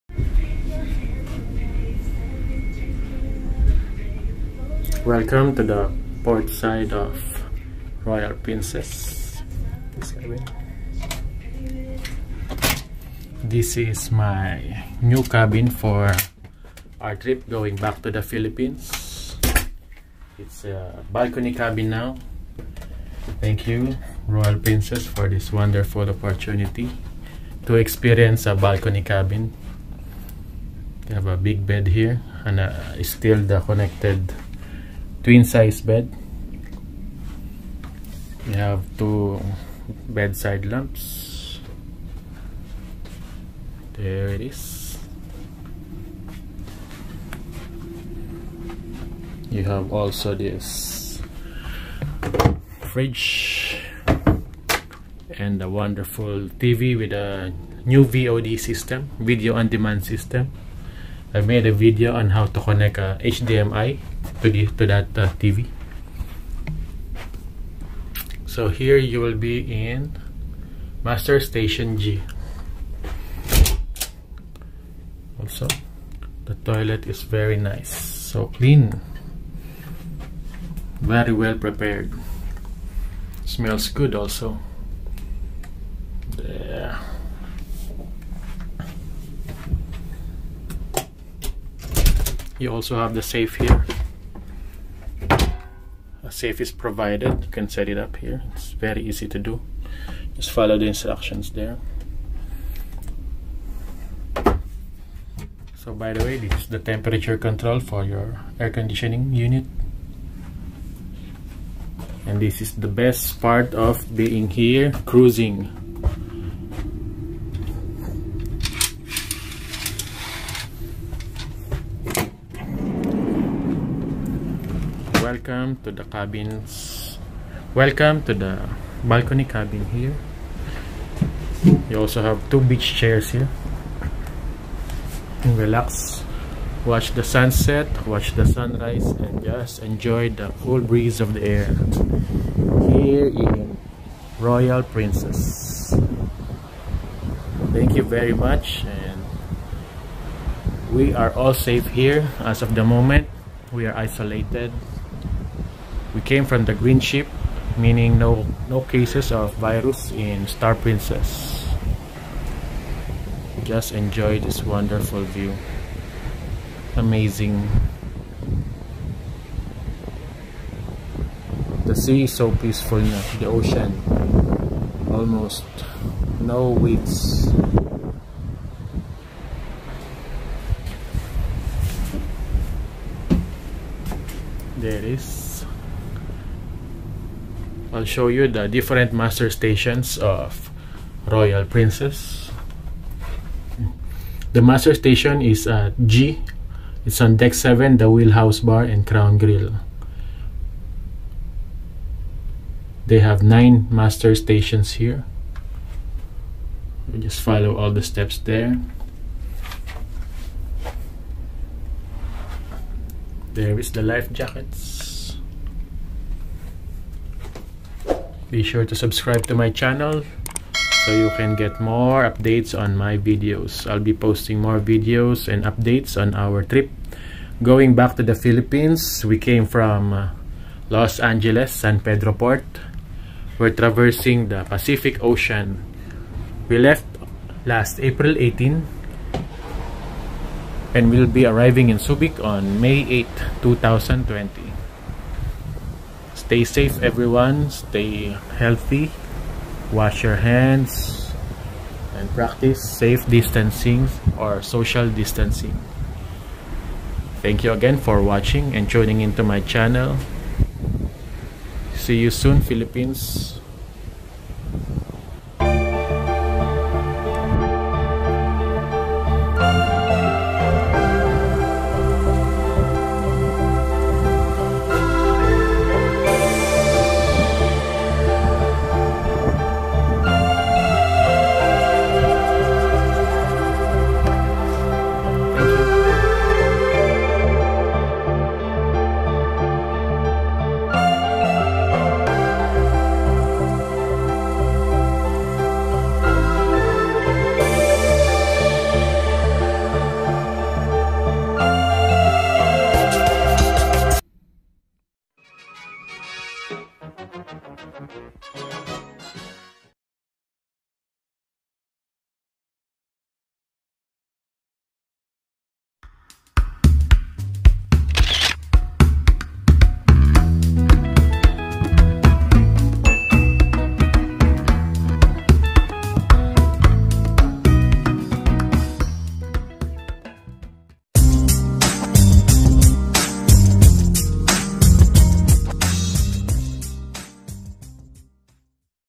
Balcony view from Royal Princess Cruises
Passenger stateroom with balcony at Royal Princess Cruises ship
Cruising from Los Angeles to Philippines for 20 days
Cruise ship passenger experience what to see and what to expect
Equipment Used:
Zhiyun Smooth-Q Handheld Gimbal Stabilizer for Smartphone
Fotopro uFO2 Flexible Tripod

Edited with Kinemaster Pro
CHECK OUT THIS PASSENGER CABIN WITH BALCONY VERY NICE